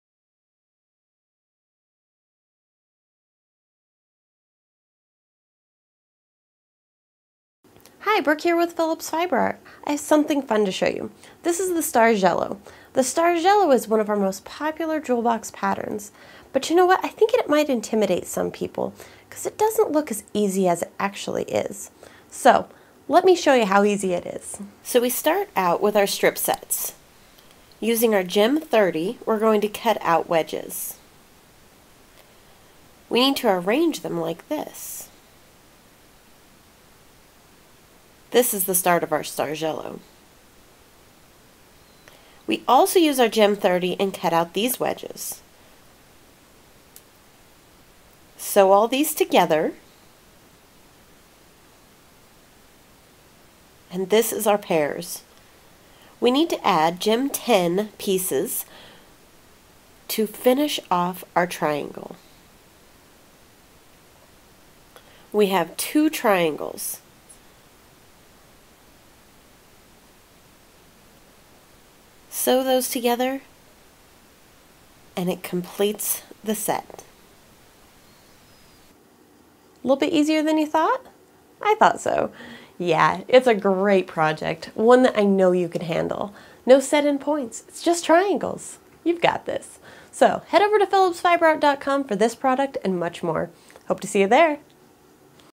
Stargello quilt using the Jewel Box Gem tools by Phillips Fiber Art
Join Brooke for a fun tutorial on the Stargello pattern, a Jewel Box favorite.